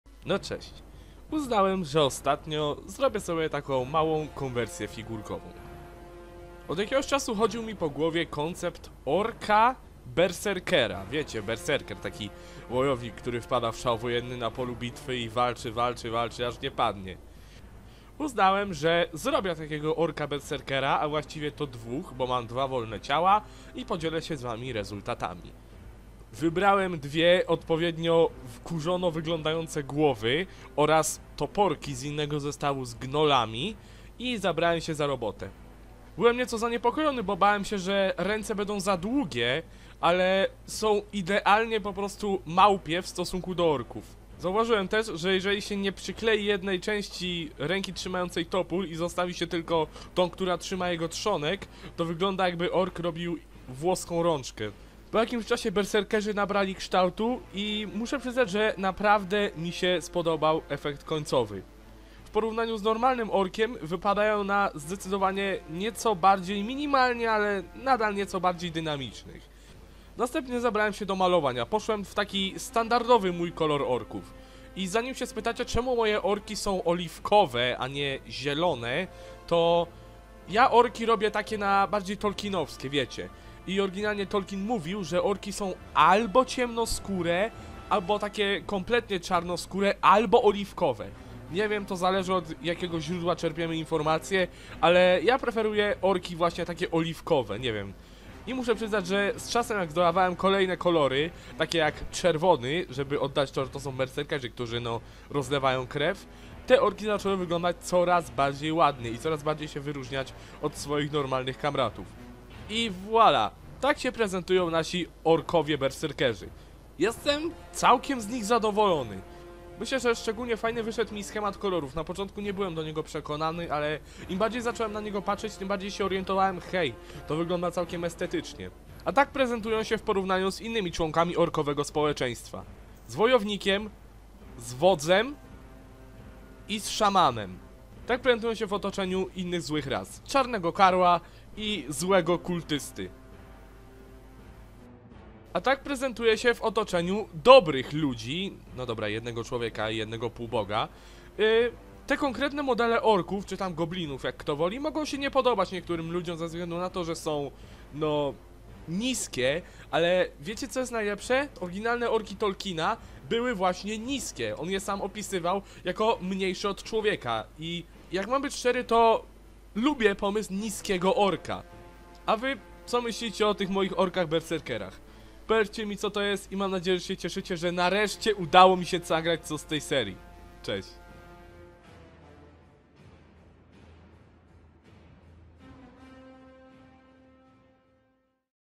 Wojti Maluje - Ork Berserker
Wojti Maluje, czyli seria gdzie prezentuję wam moje hobby malując figurki do gier tabletop. W tym odcinku robię kitbash Orków Berserkerów.